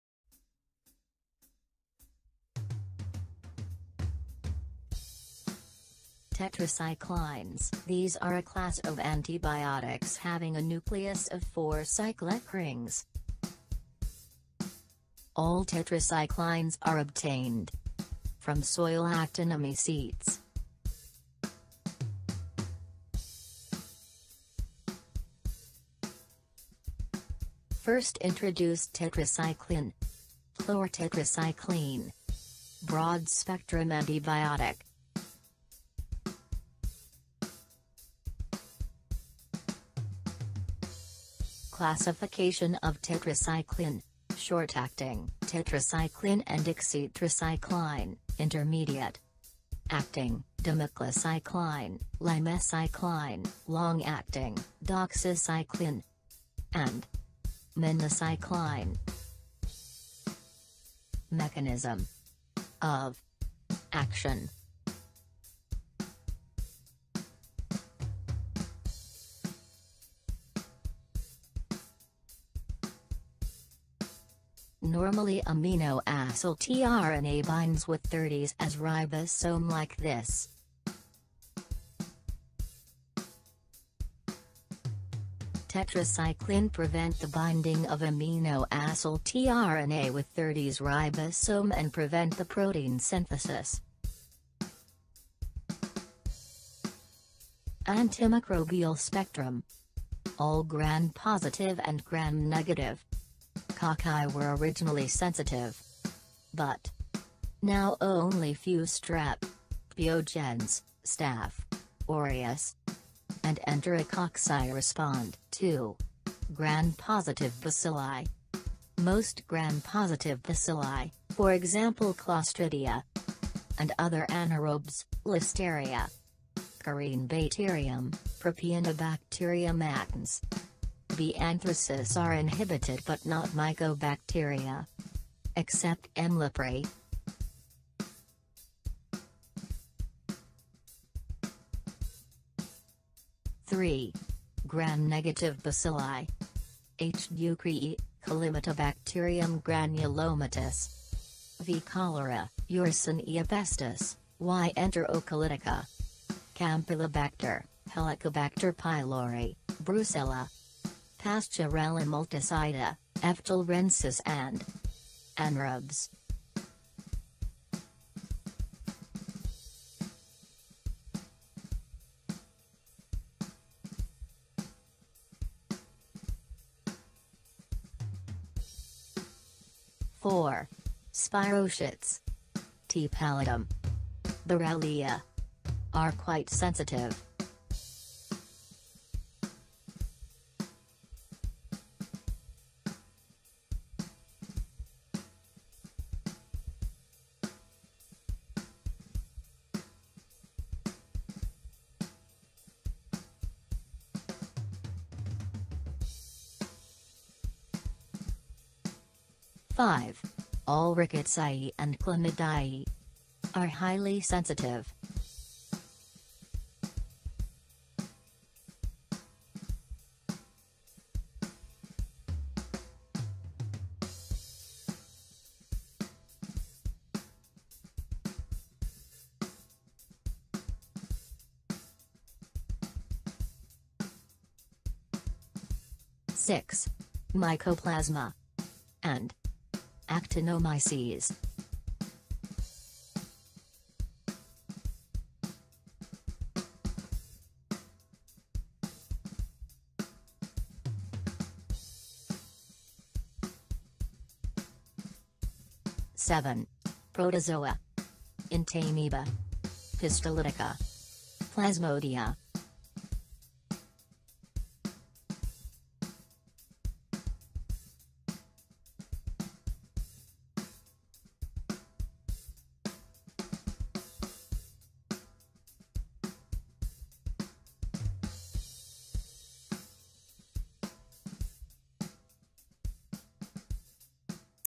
Tetracyclines Drug Introduction and Mechanism of Action
Pharmacology Video
Tetracycline Antibiotics
Classification of Tetracyclines
Introduction and Mechanism of Action